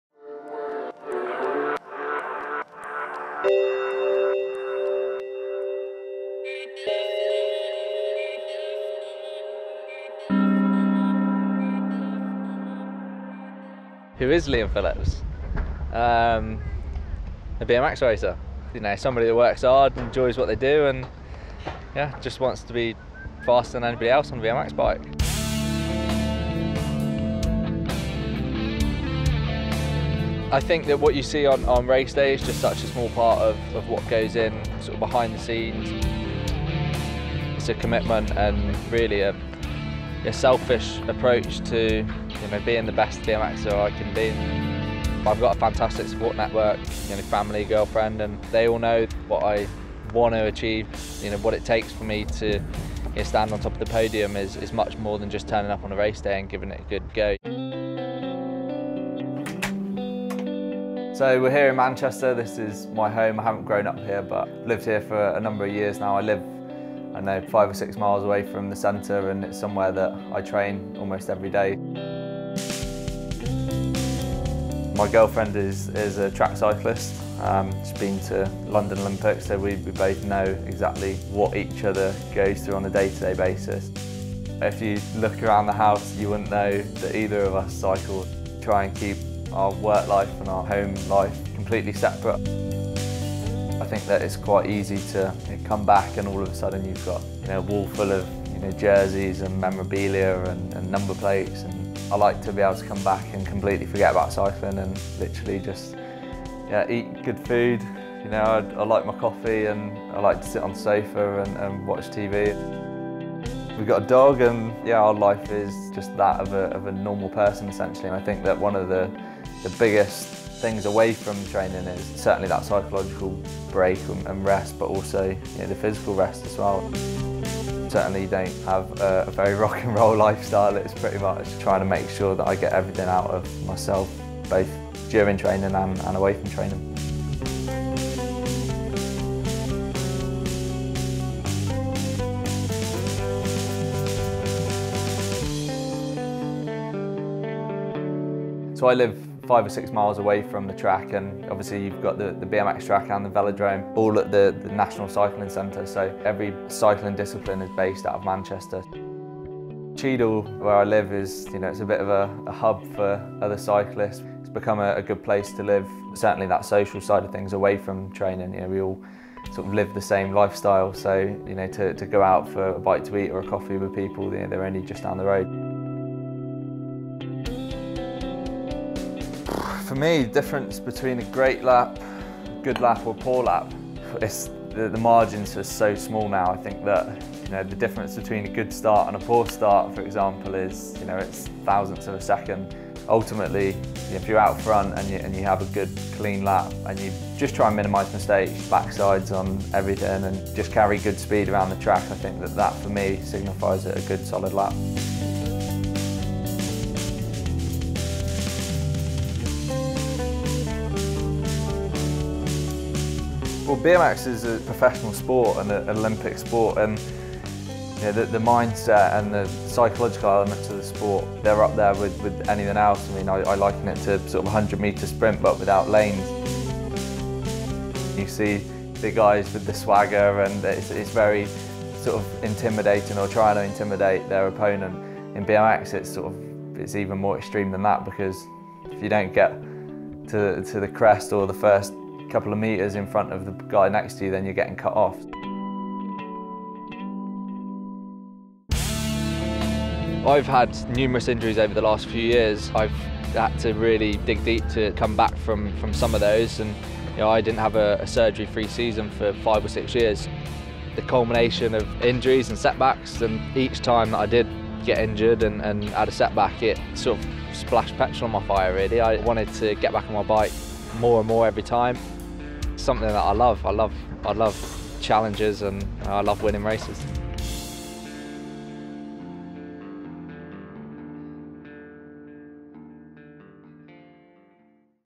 Tioga Pro:File - Liam Phillips
Pro:File of Tioga rider and overall Supercross Champ Brit Liam Phillips.